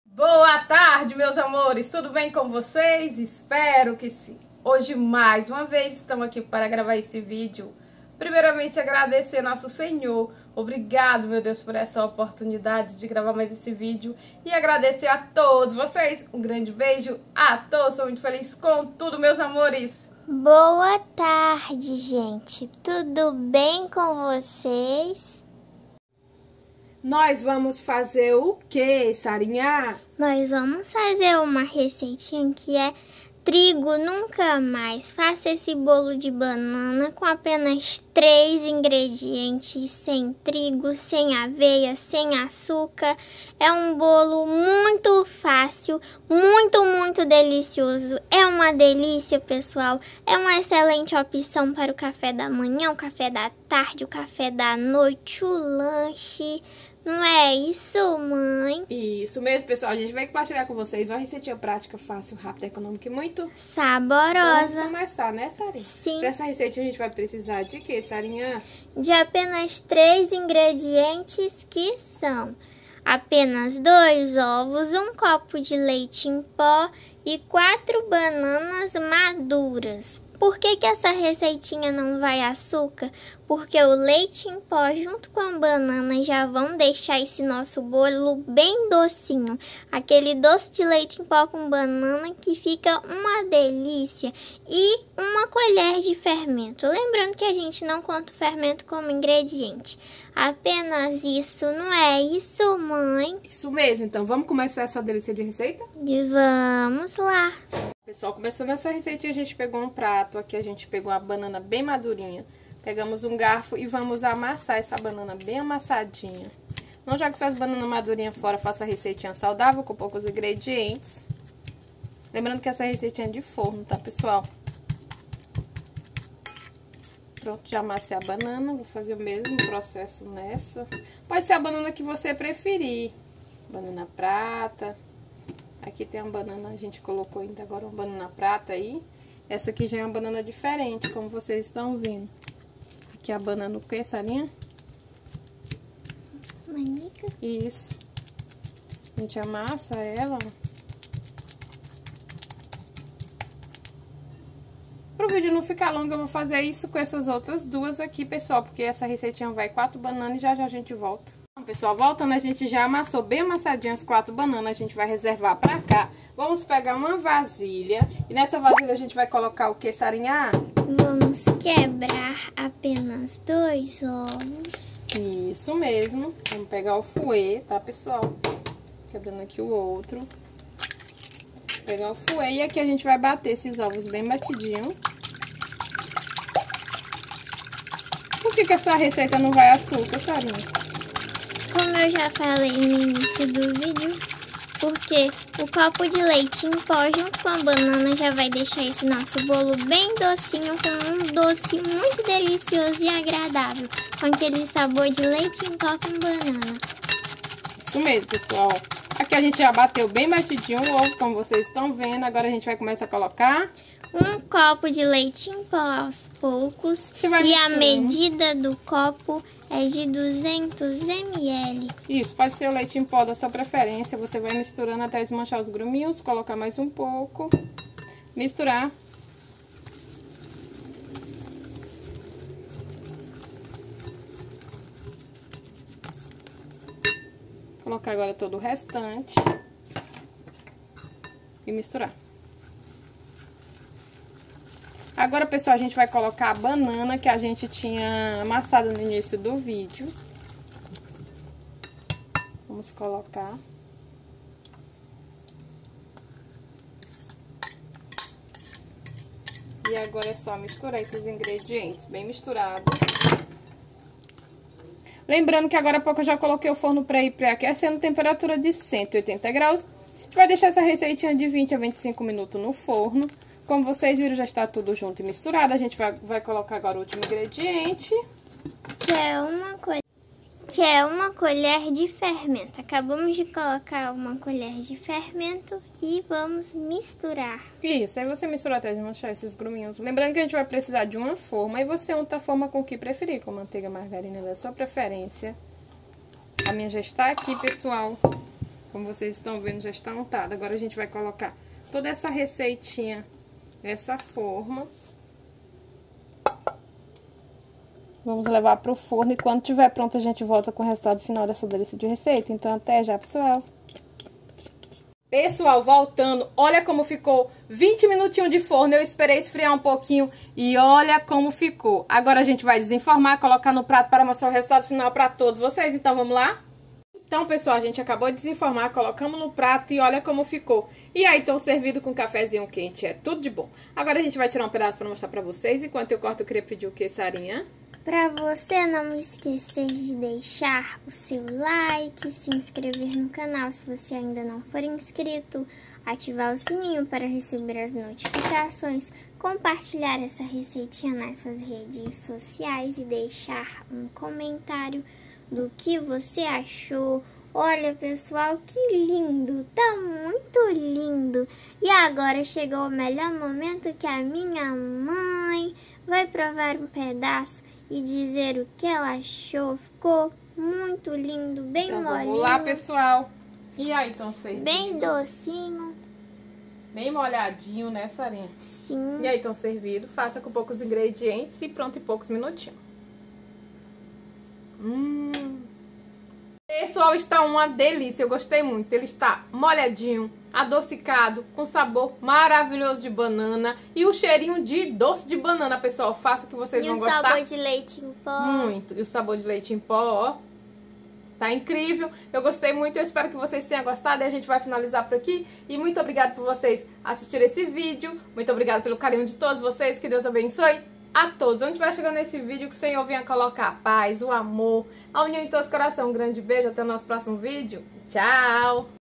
BOLO DE BANANA DE 3 INGREDIENTES-SEM TRIGO,SEM AVEIA,SEM AÇÚCAR PARA O CAFÉ DA MANHÃ DELICIOSO
ingredientes:
2 ovos
1 copo de leite em pó
4 bananas maduras

1 colher de fermento